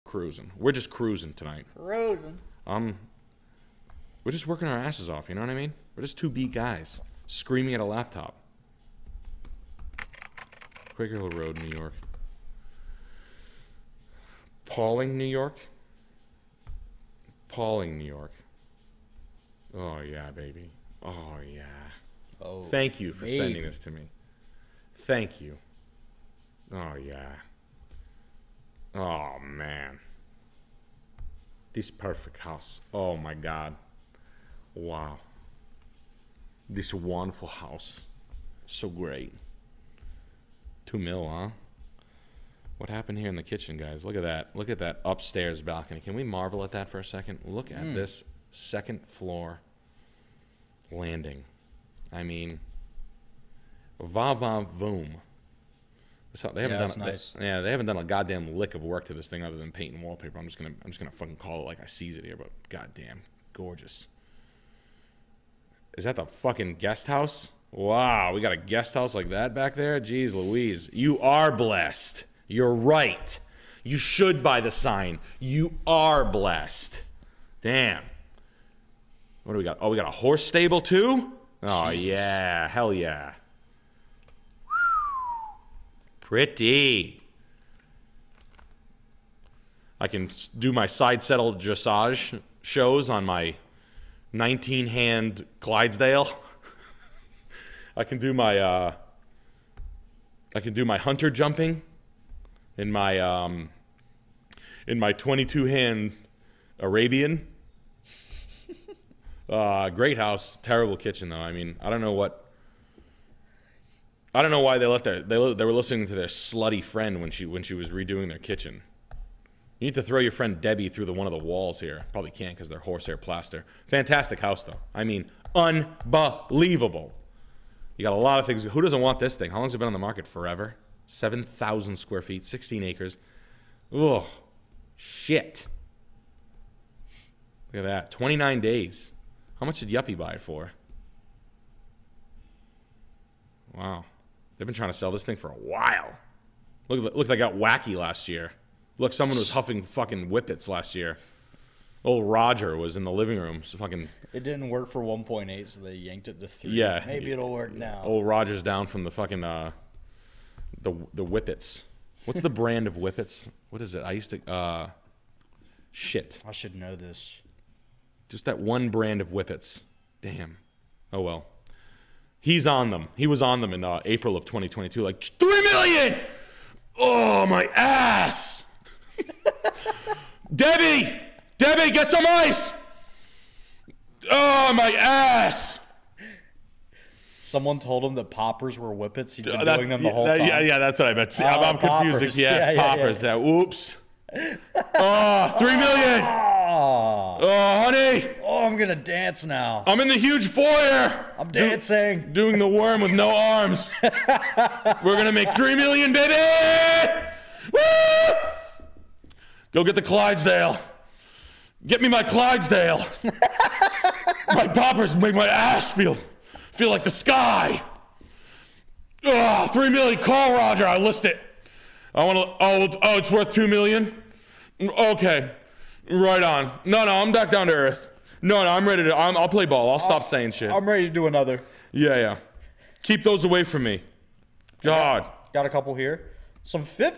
Nick Likes This House But Not For That Price!
Nick really likes this house but finds out the it's overpriced.